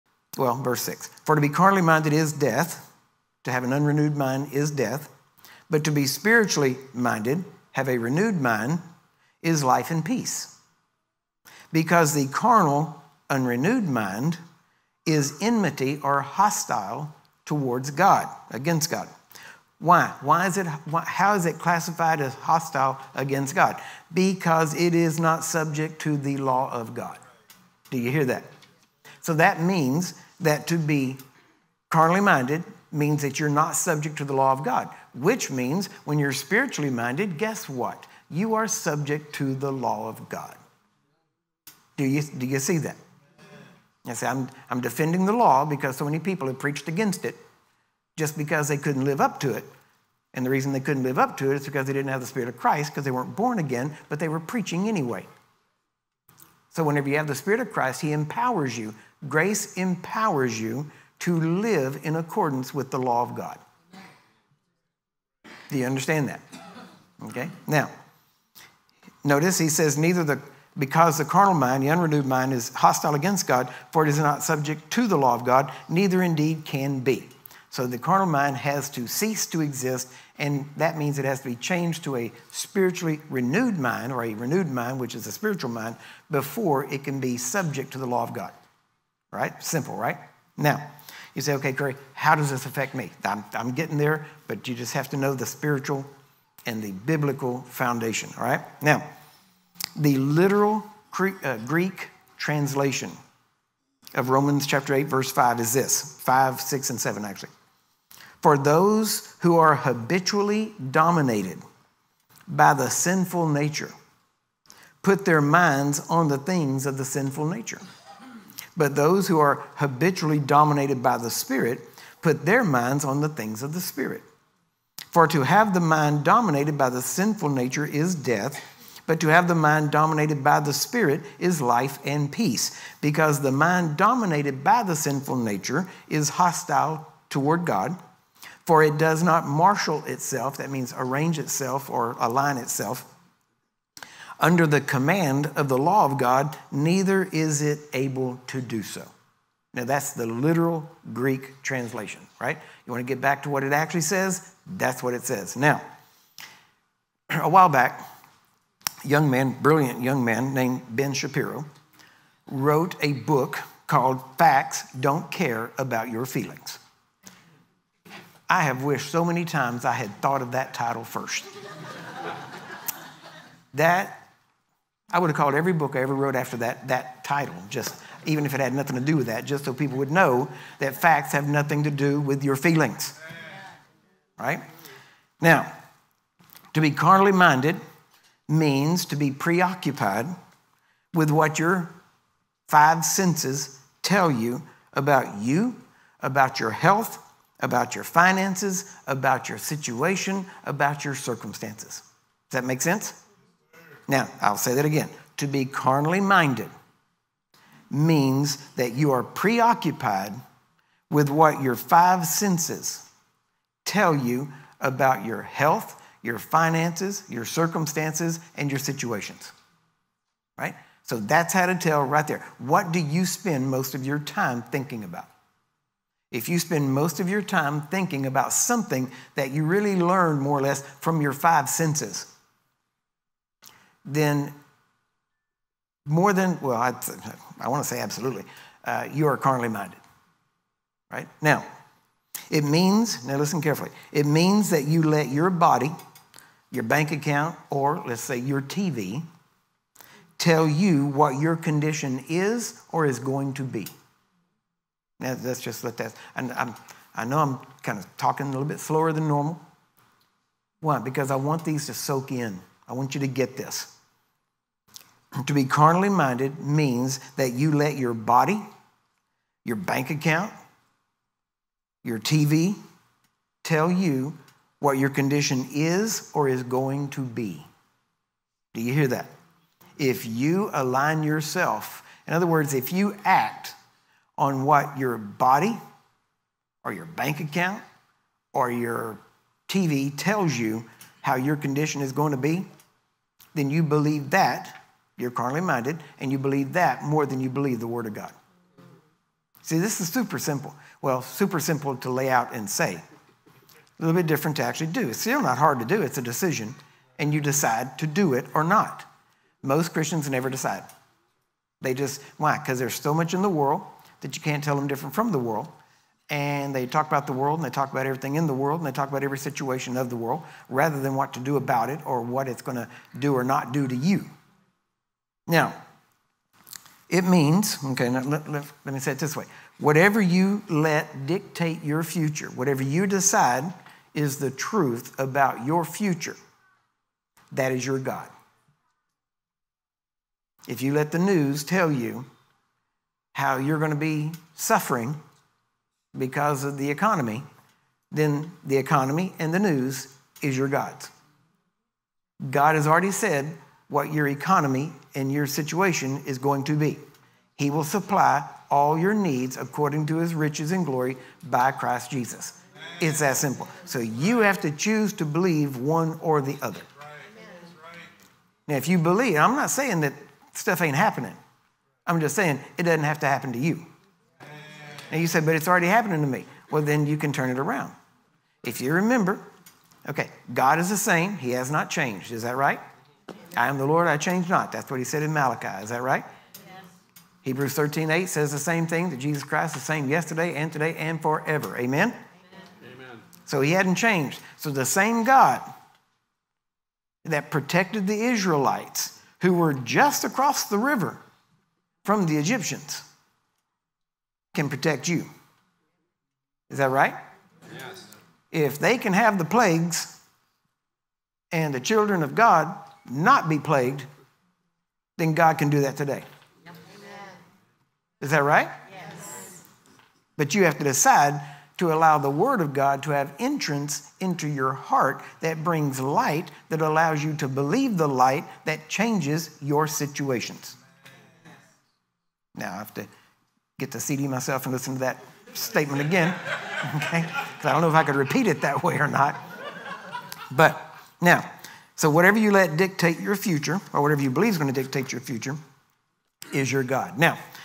What is your mind like? Curry Blake
In this video, Brother Curry Blake explains the substantial difference between being of a carnal mind and a spiritual mind and what the consequences are.
Understanding this difference is key to changing in the direction of being like Jesus.